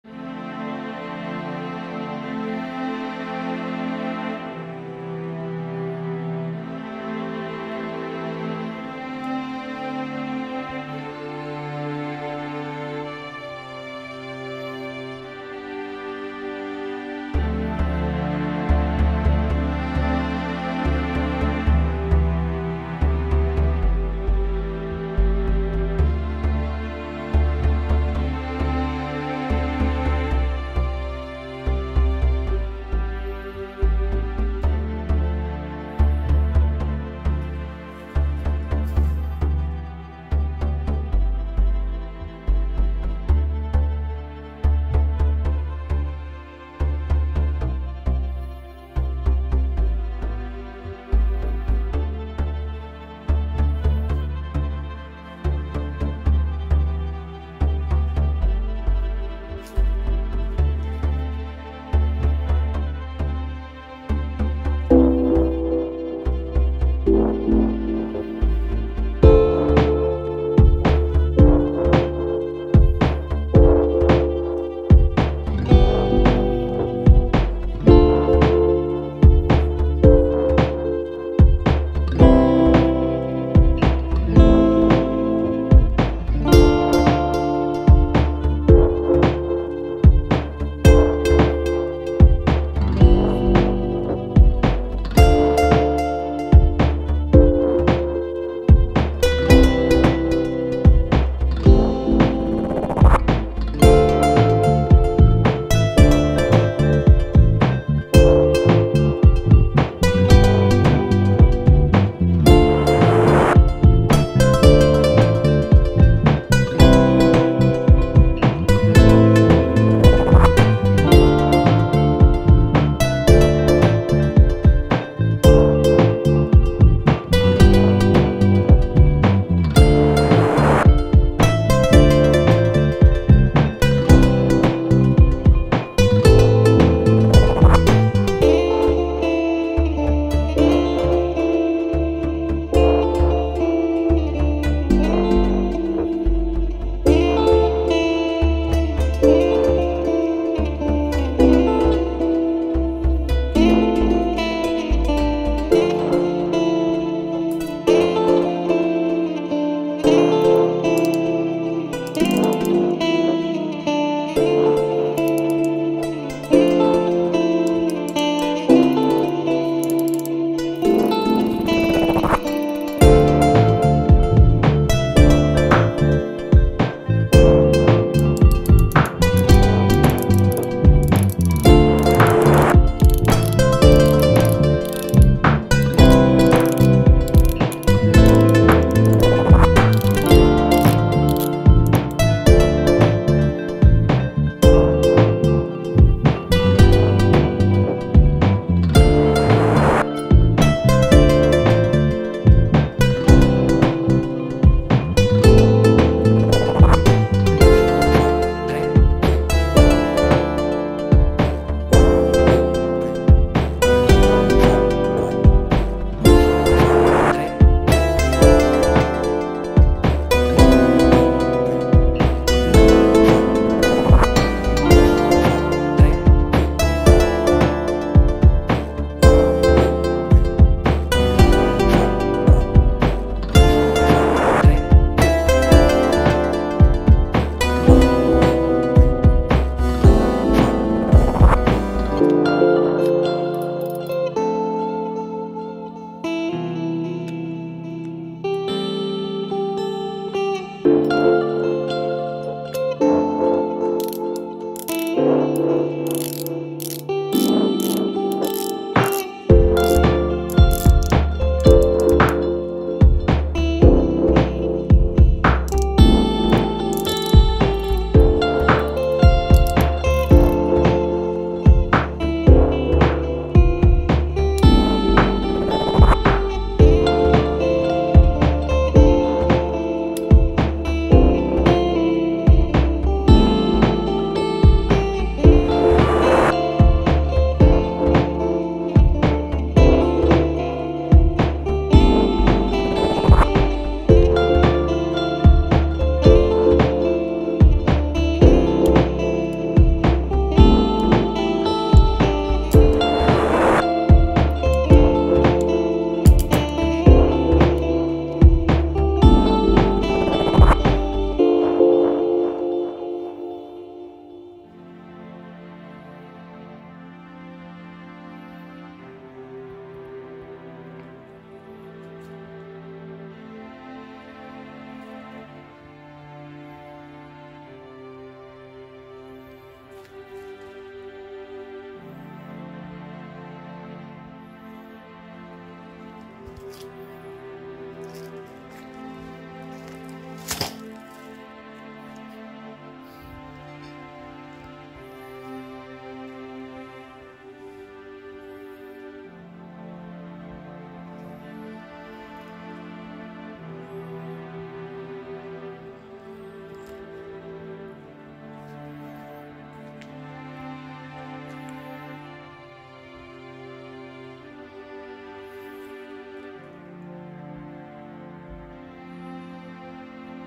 Shimano ultegra CI4+14000 XSC
A Shimano ultegra XTC után bemutatom az XSC változatot is. Az egyetlen különbség a színben van, de szerintem ezt már sokan tudják.
After the Shimano ultegra XTC, I also present the XSC version. The only difference is the color, but I think many people already know that.